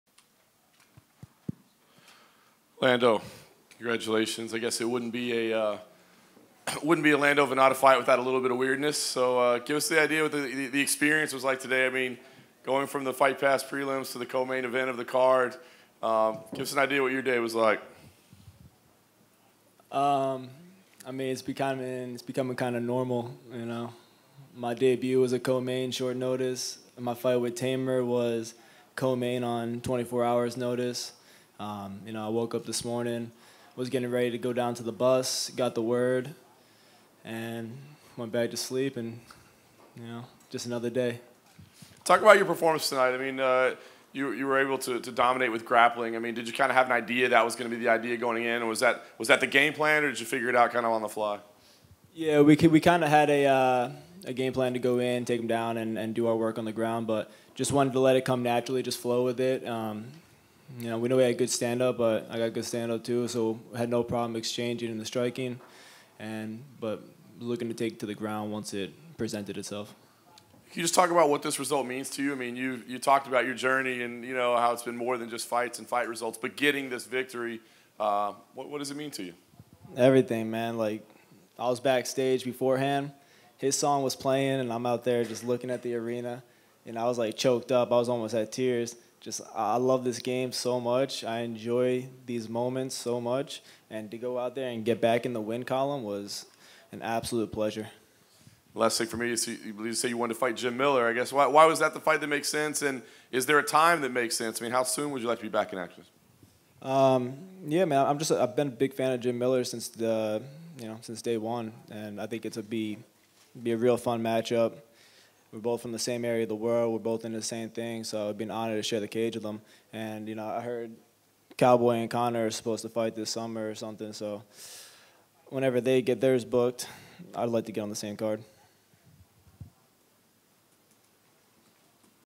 UFC 234: Lando Vannata Post-Fight Press Conference - MMA Fighting
At the UFC 234 post-fight press conference, Lando Vannata discusses his win over Marcos Mariano, learning that he was going to be in the co-main event and more.